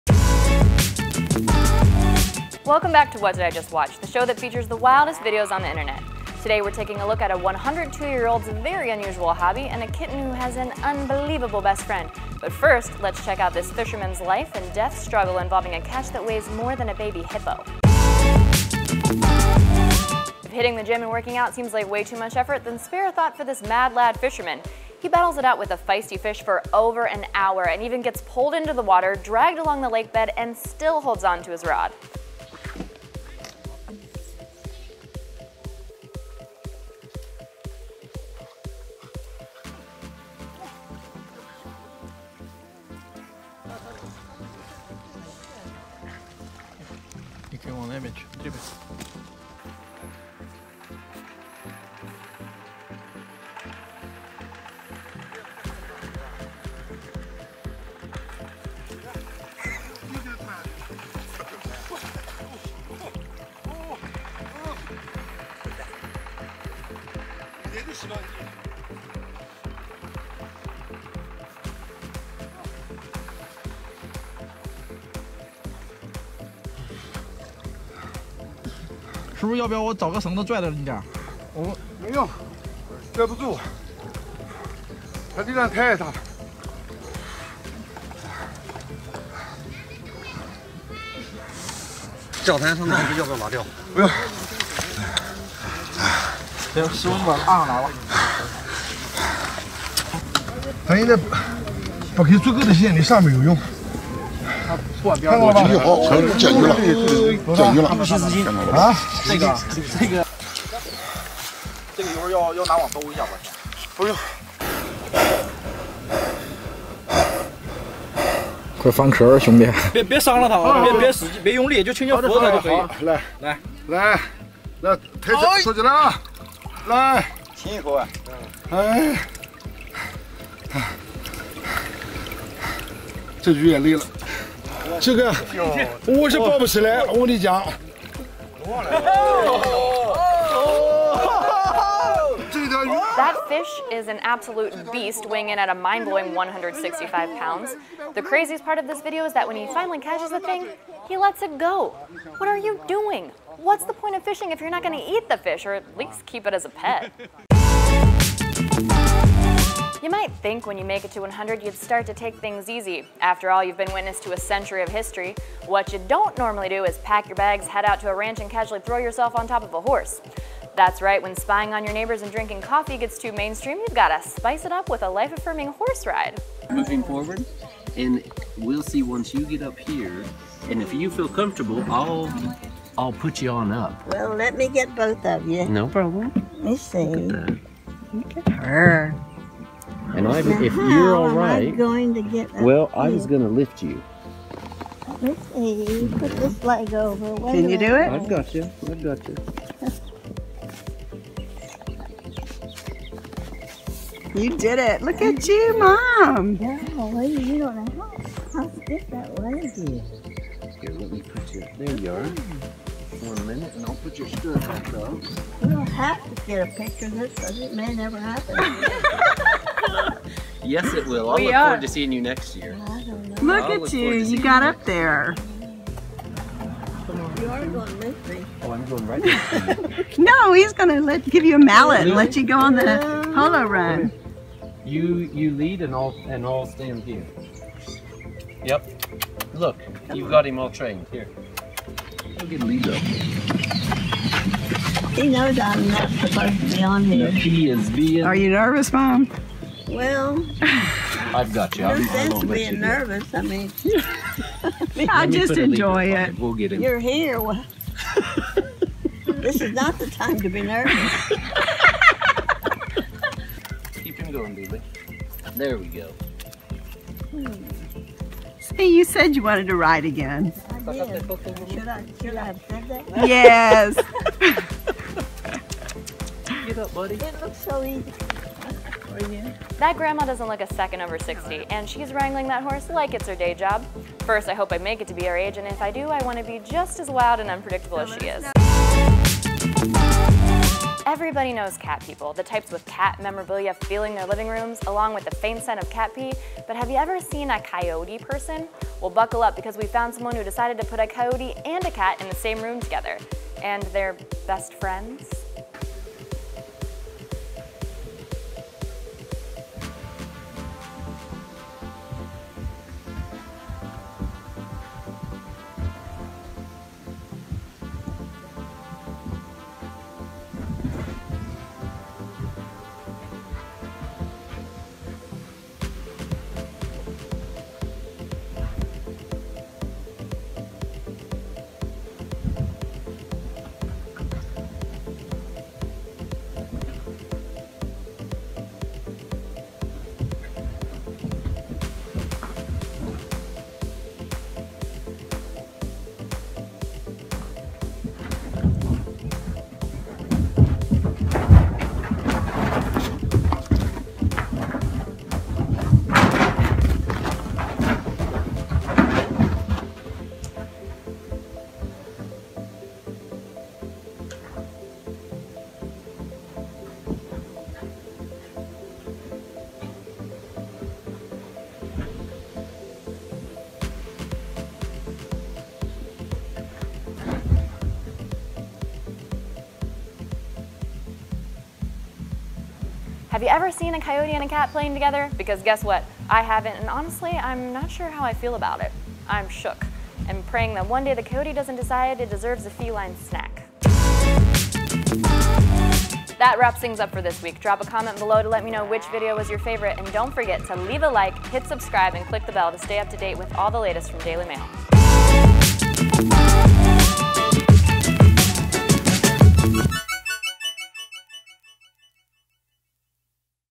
HIPPO FISH, GRANNY ON HORSEBACK AND UNBELIEVABLE FRIENDS - WDIJW #13
Welcome back to What Did I just Watch, the show that features the wildest videos on the internet. Today we’re taking a look at a 102-year-old’s VERY unusual hobby and a kitten who has an unbelievable best friend. But first, let’s check out this fisherman’s life and death struggle involving a catch that weighs more than a baby HIPPO.

Hosted by: Stephanie Haney

Credit: